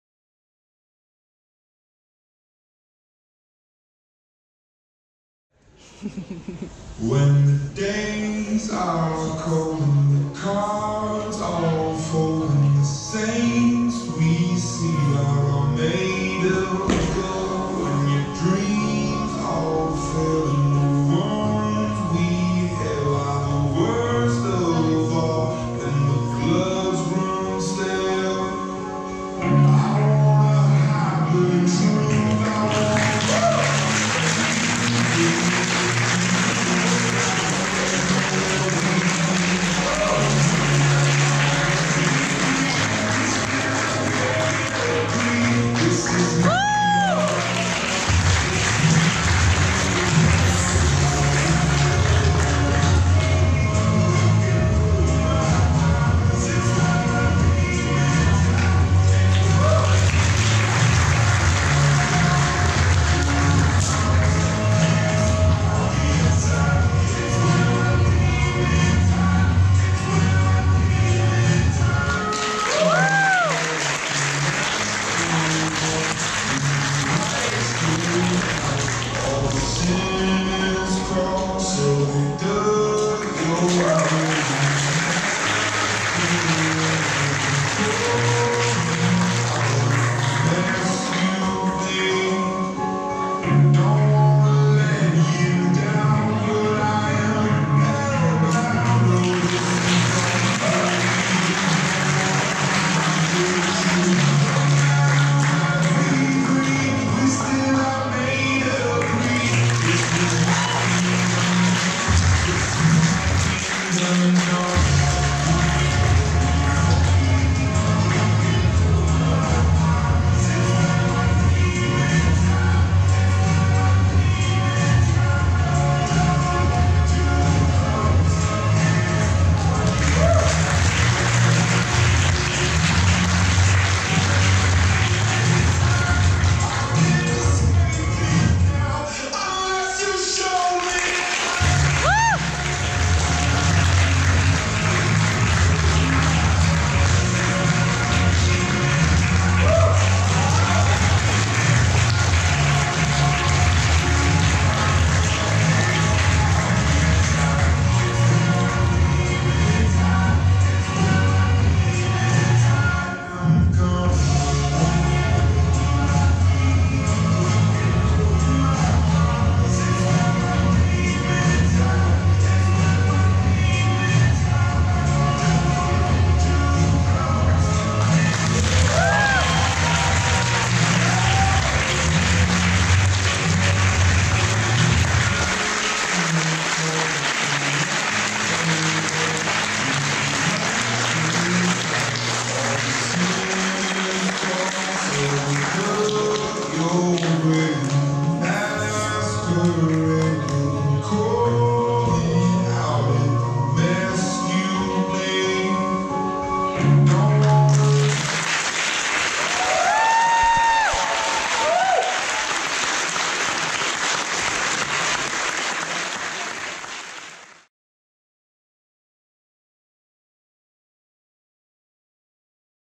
Zakončení podzimního semestru 2017/18 - Pole Dance Teens - Trio Friends
Vystoupení v rámci večírku k zakončení podzimního semestru 2017/18